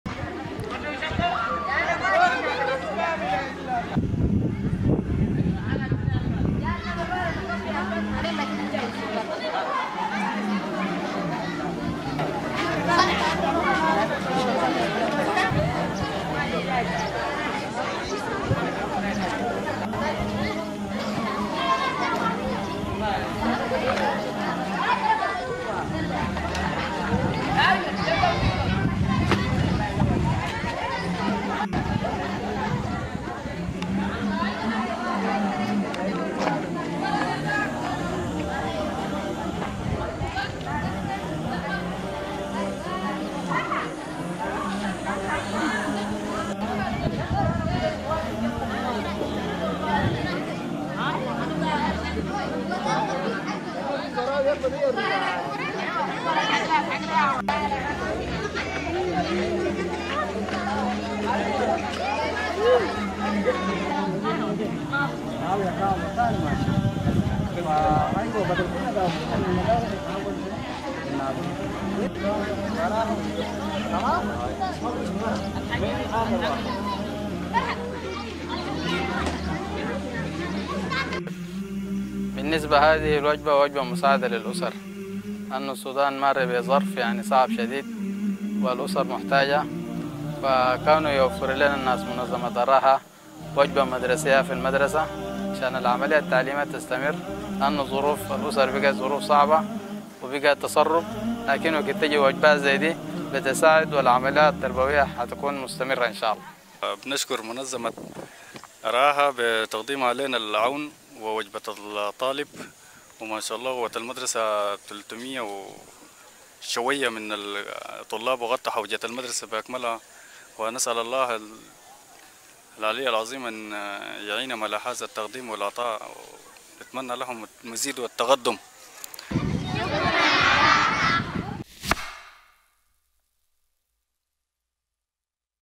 Distribution of Fortified Rice at an Elementary School
Distribution of 200 boxes of fortified rice from our partner Feed My Starving Children in Aljeerah elementary school in Allaffa village.